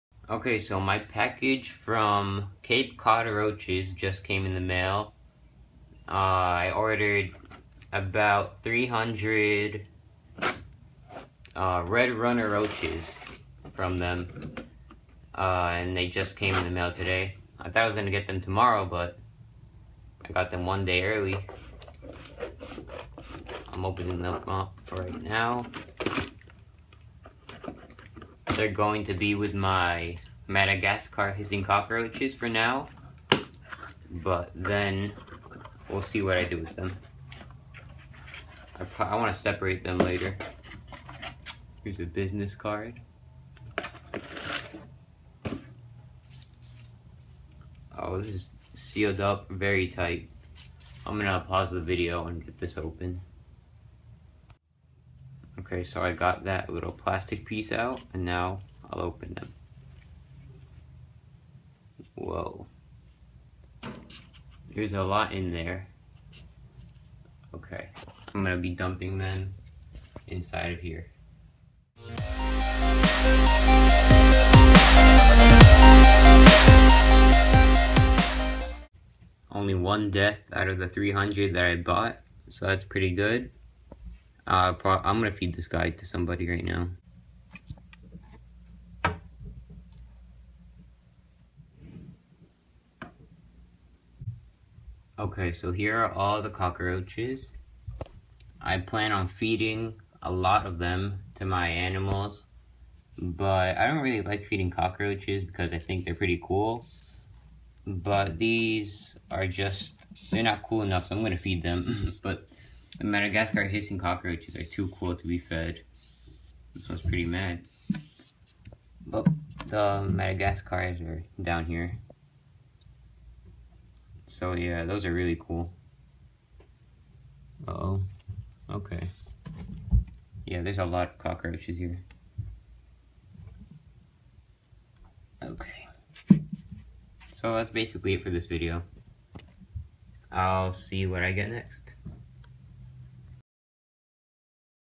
300+ Red Runner Roach (Turkestan Cockroach) UNBOXING - Cape Cod Roaches
300+ Red Runner Roach (Turkestan Cockroach) UNBOXING - Cape Cod Roaches